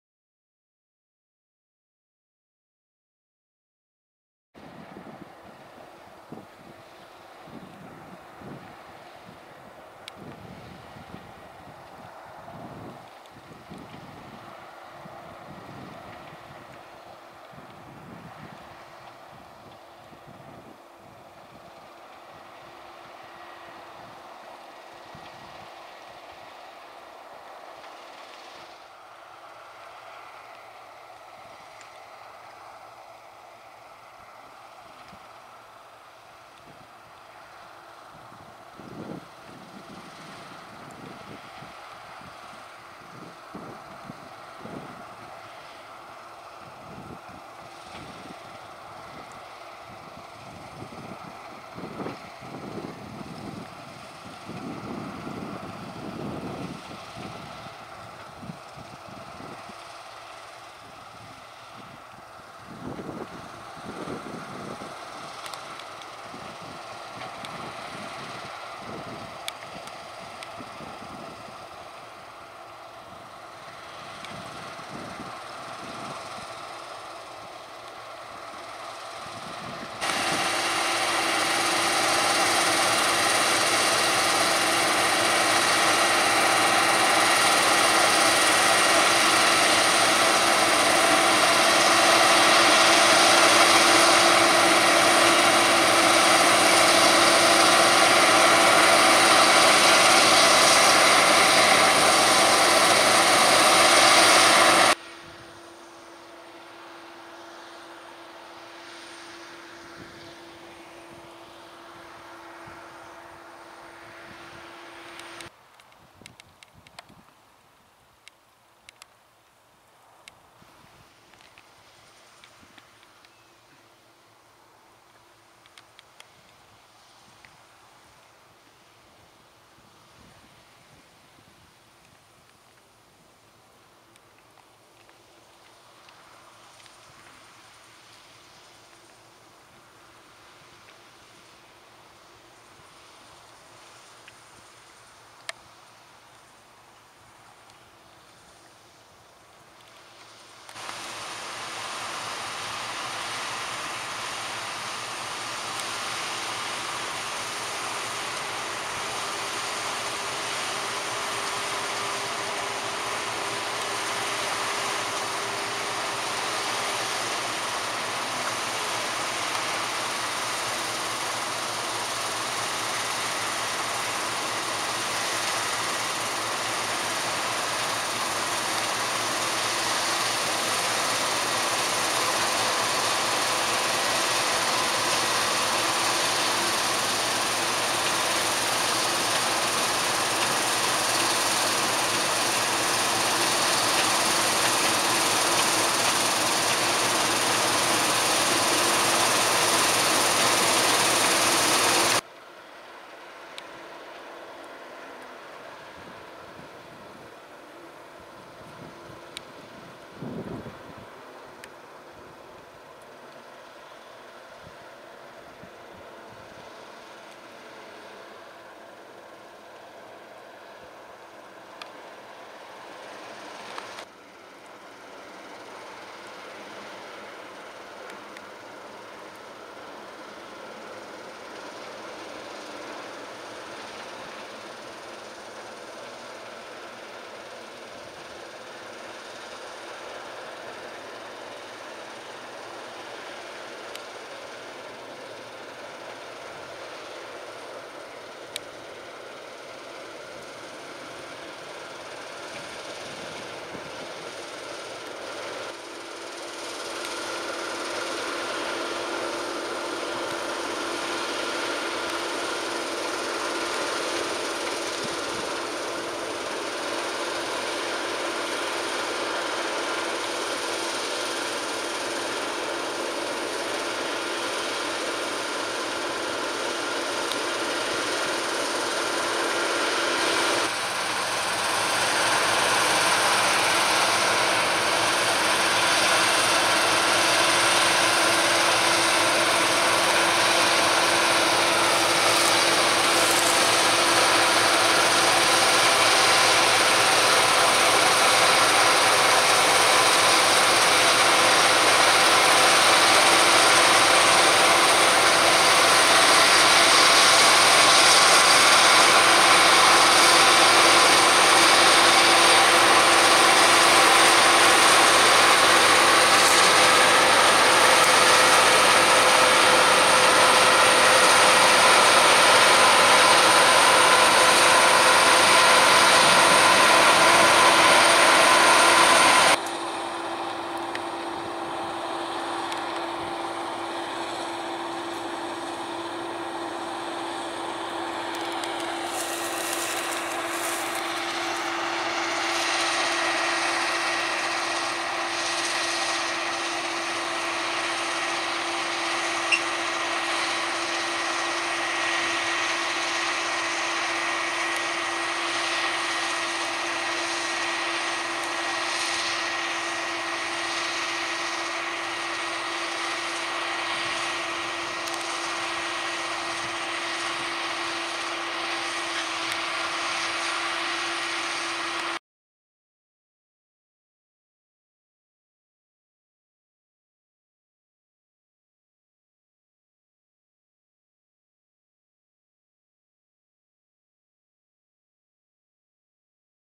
Harvest 2022 - Combining Barley with Claas Lexion 660 & John Deere 6195R carting
Taken back in August and you've already seen the baling outfit, 7R 330 & Krone (see end screen). Few evenings before after work I managed to get some of the combining, which was near completing for the evening. Farms outfit combining spring barley with their Claas Lexion 660 and carting with their John Deere 6195R. With thaks to all involved. Filmed in Hampshire, UK. Watch in HD for best quality.